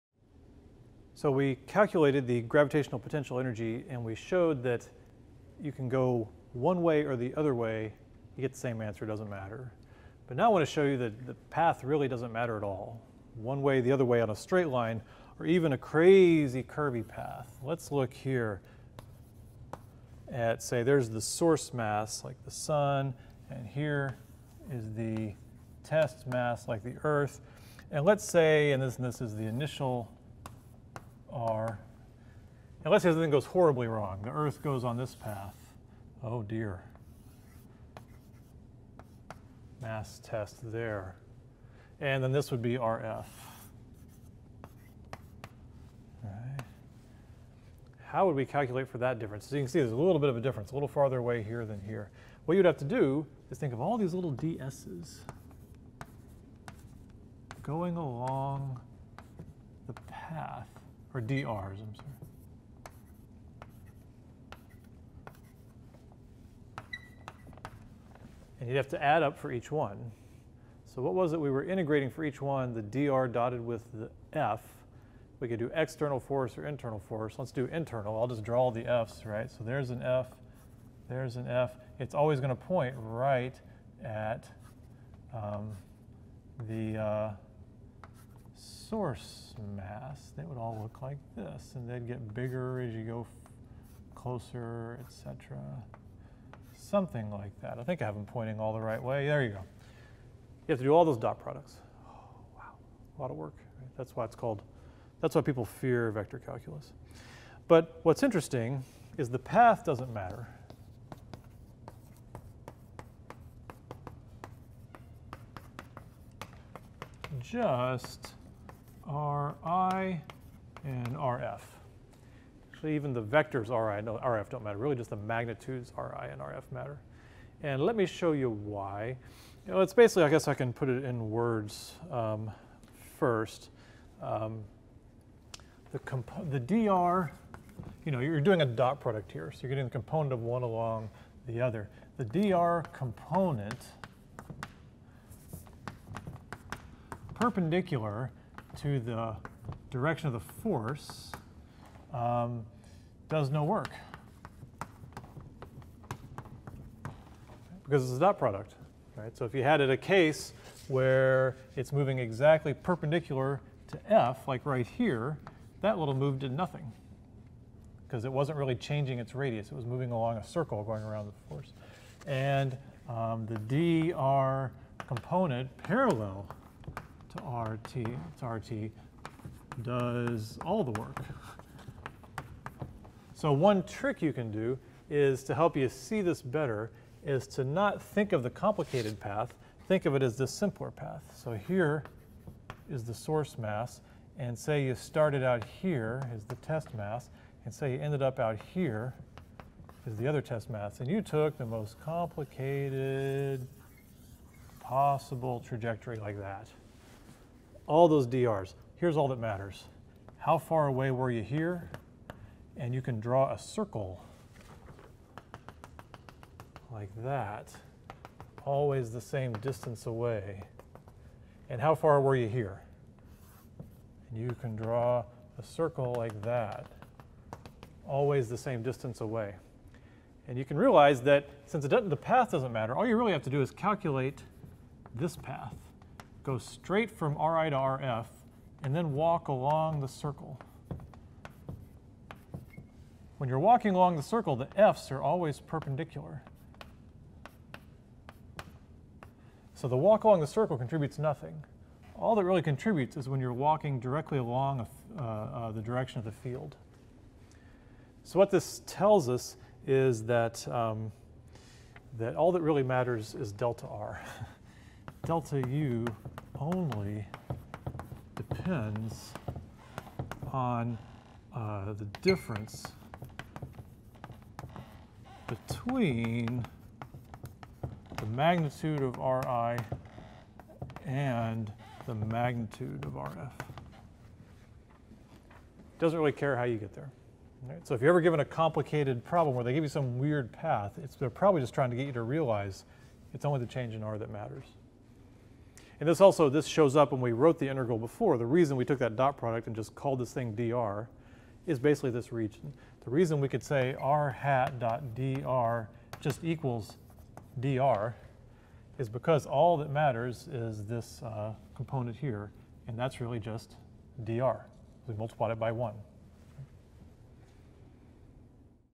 PHYS 101 | Gravitational Fields 3 - Gravitational Potential and Path
In this lecture we discuss the lack of path dependence of the potential energy in a gravitational field.